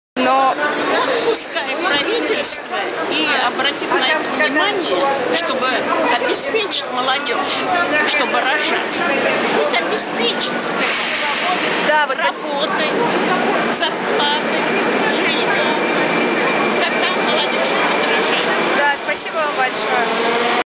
Участница о сути акции "Что-то не так? Рожайте" в метро
Участница о сути акции "Что-то не так? Рожайте" организации "Старшее поколение" 18 мая 2011 года. Фотографии